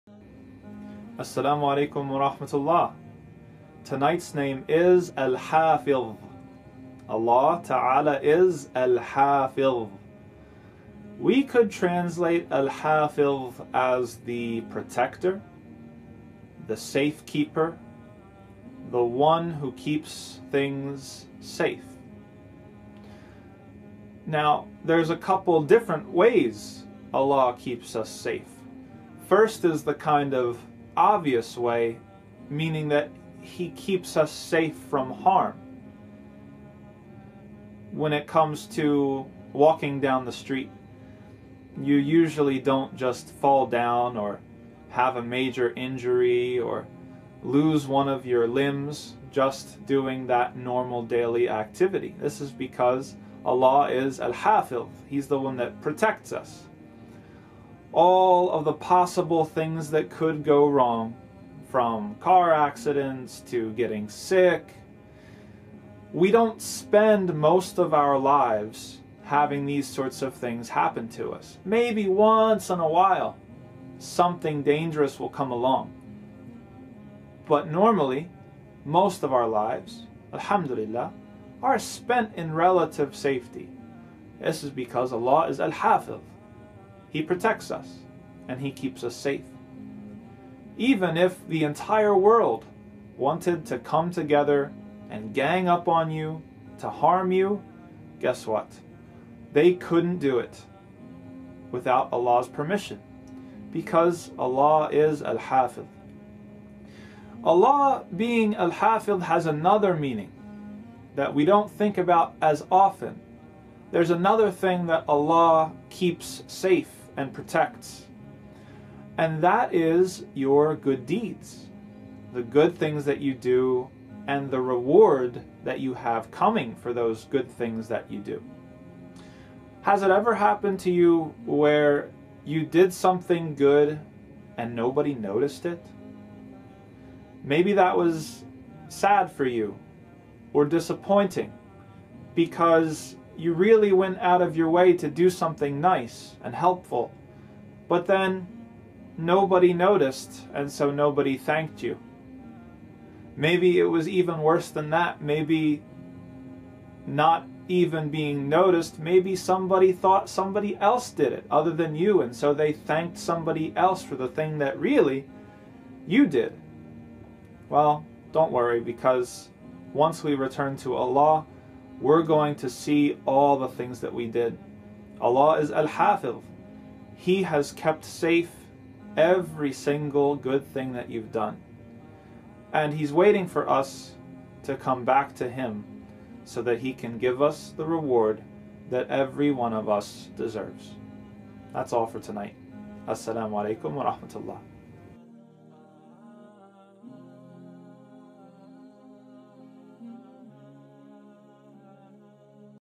99 Names of Allah - Al-Hafidh
Countdown to Ramadan 2021 with MCAMV as we go through the 99 names of Allah. This series is aimed towards the youth as a simplified introduction into the most beautiful names of Allah.